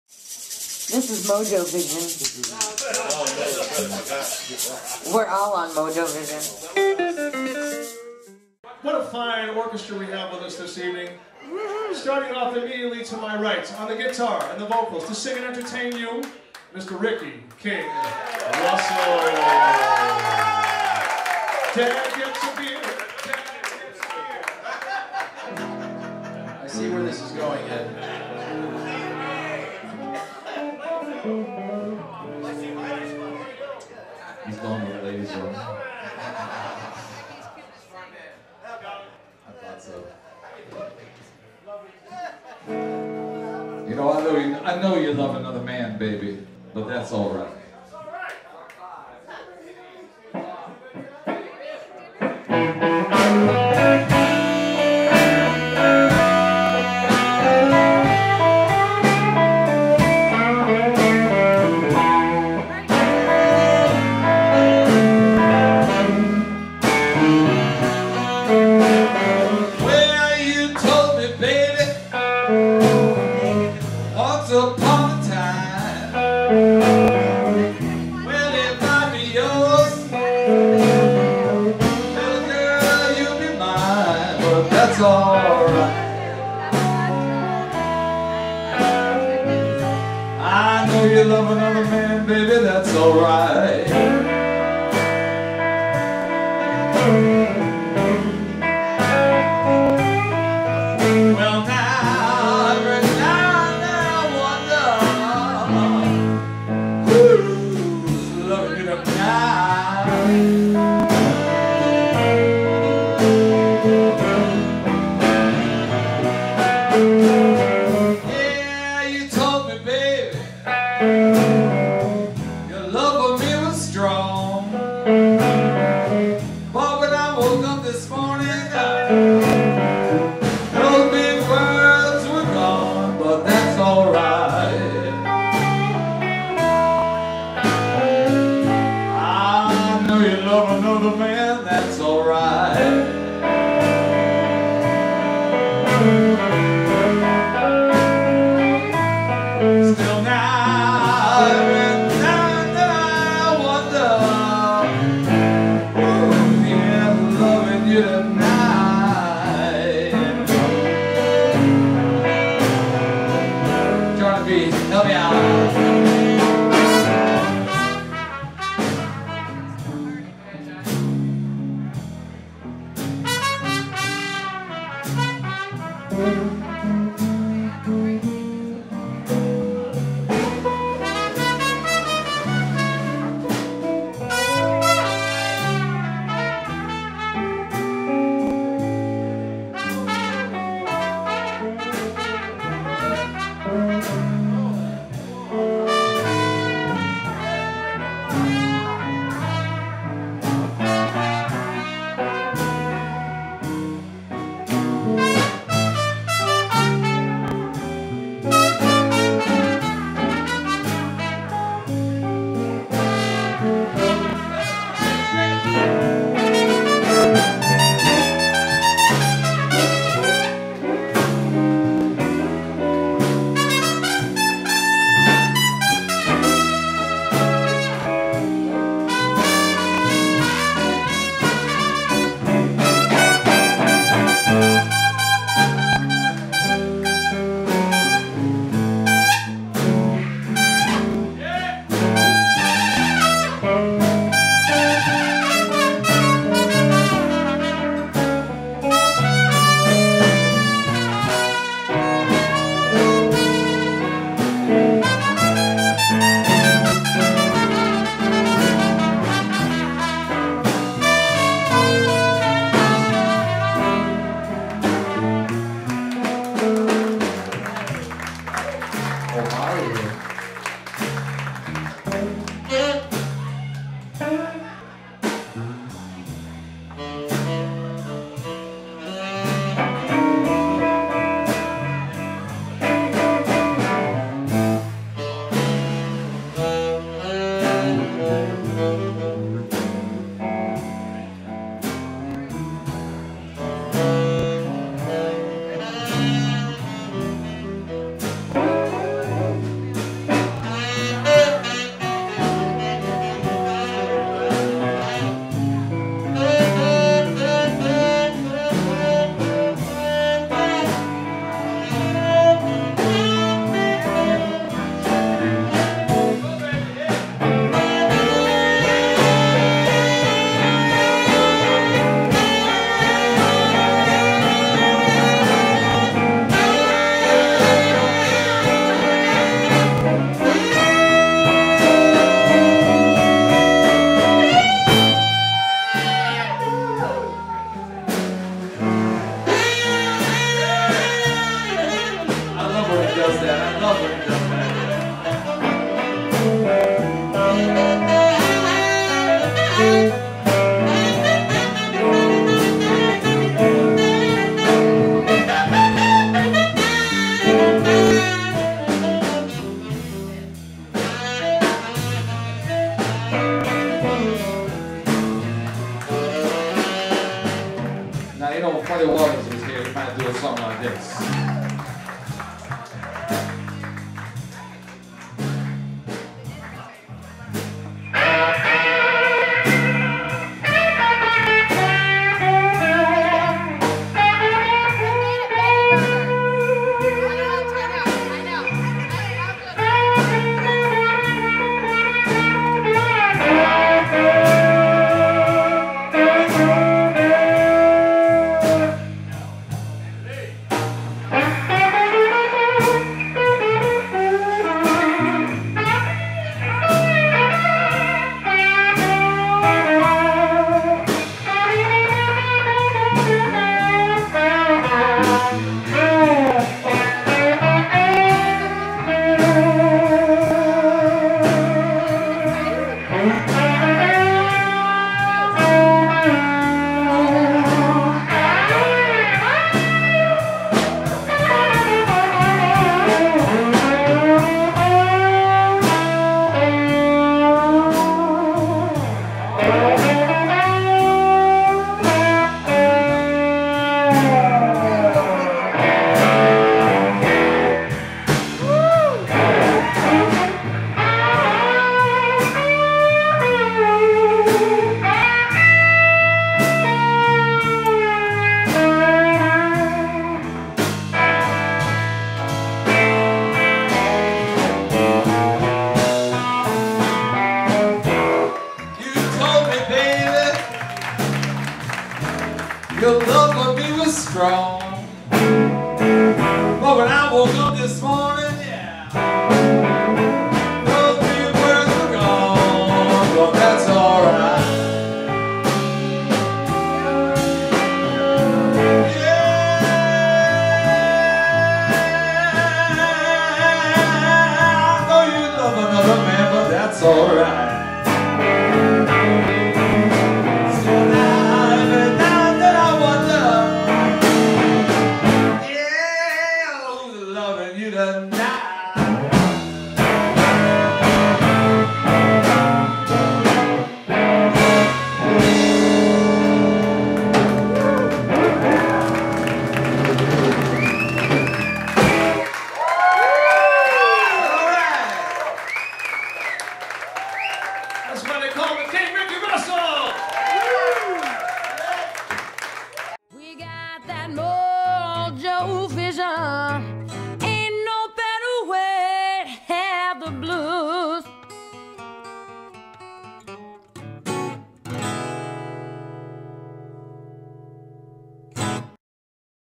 Eddie Scheer's Tuesday Night All Stars Live @ The Corrib Pub 6/30/15
NATE DOW'S PRE- BIRTHDAY BASH with ED SCHEER, RICKY "KING" RUSSELL, MARIO PERRETT, JOHNNY BLUEHORN, RANDY BRAMWELL and STEVE BROWN